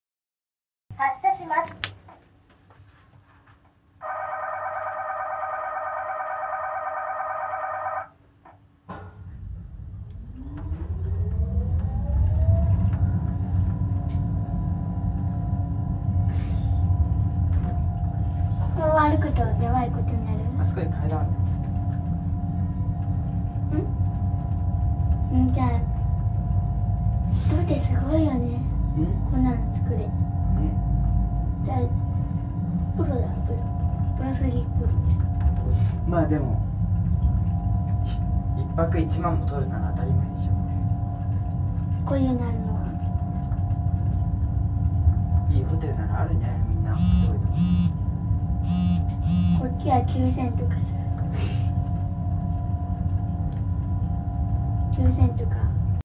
焼津グランドホテルの上り坂リフト
子供と一緒に行って、乗り取りしました。